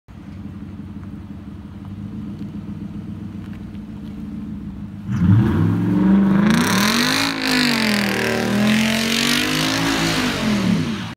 2014 F150 5.0 Borla ATAK exhaust
Feb, 2017 Boston, MA Winter storm. I decided to have a little fun. 2014 F150 5.0, Borla ATAK exhaust and Omega Tune